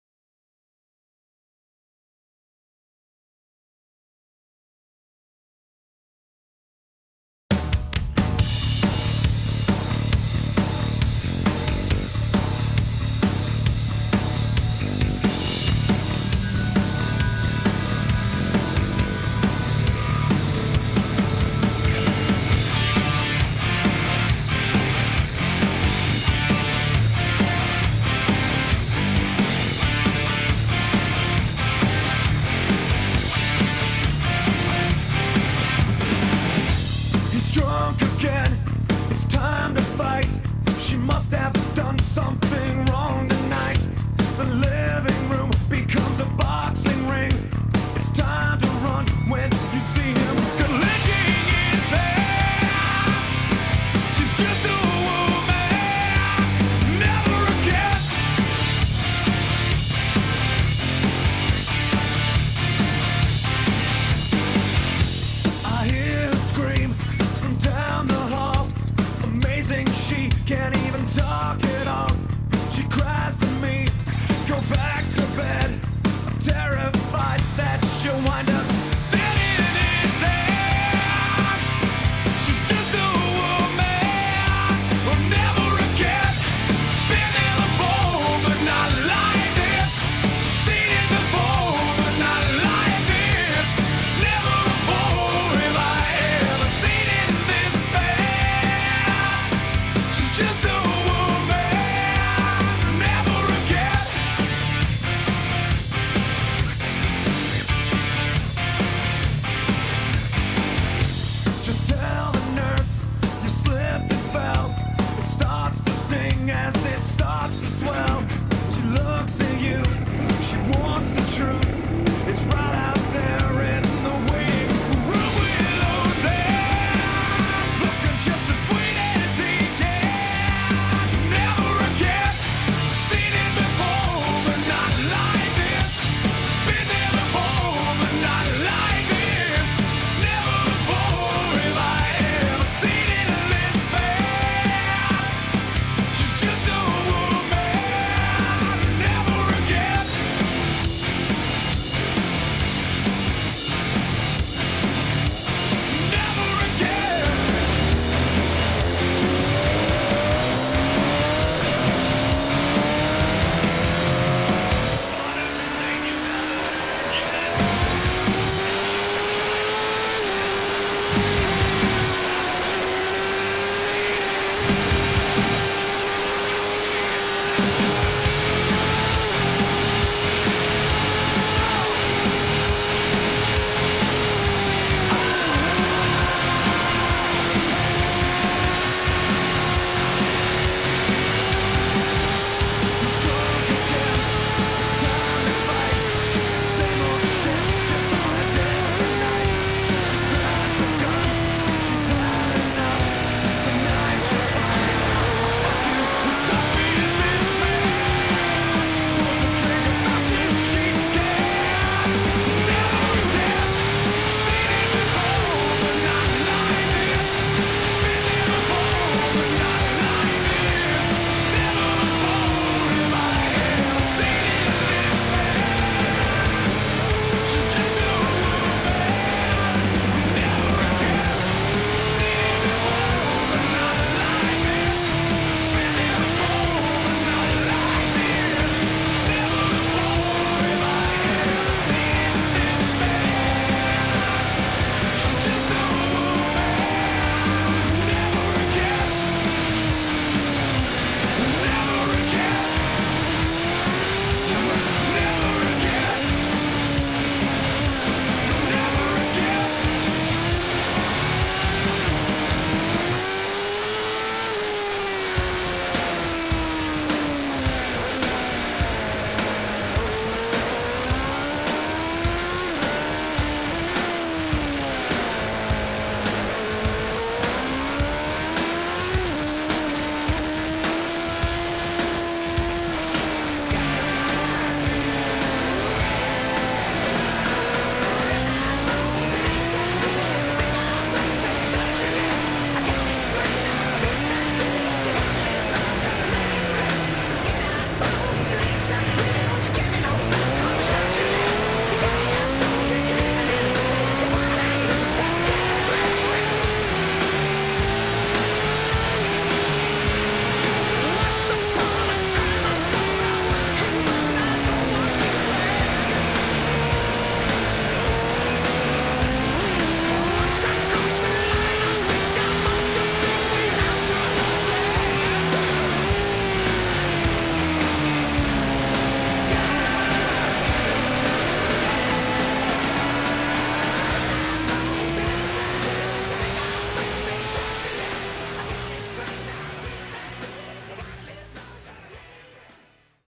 MACAU 2004 Onboard Video MV AGUSTA SPR best video
SBKGP.com and RXR team at Macau 2004
Rider: Umberto Rumiano
Mechanical: Mauro Ferretti
Team Manager: Stefano Spiti